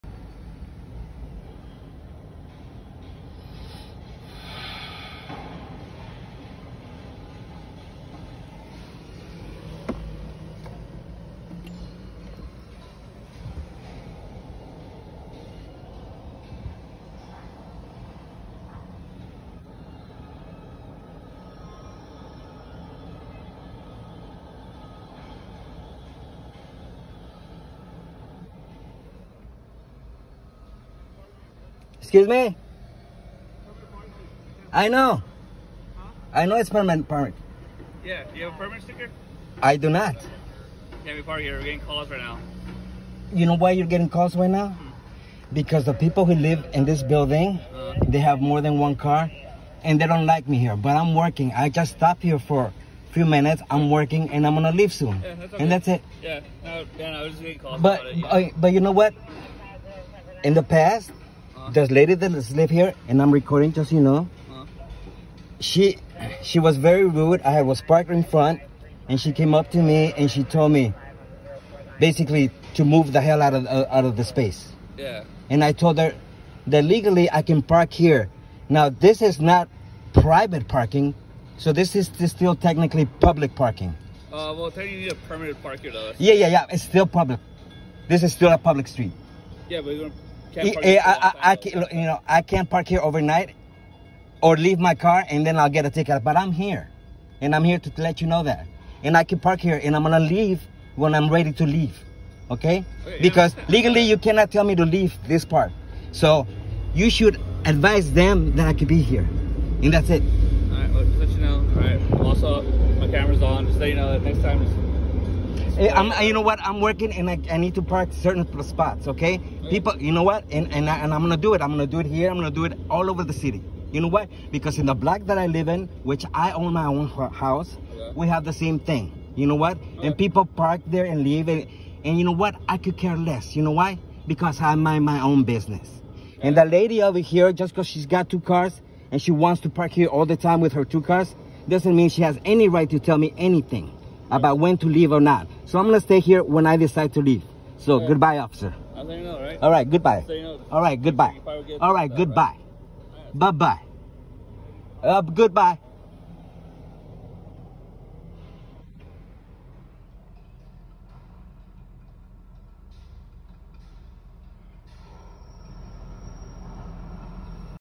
Cops tries to bully me out of parking spot, because Racist Karen called. Part 2
Cop attempts to bully me out of parking spot because racist Karen called but gets owned and does the walk of shame.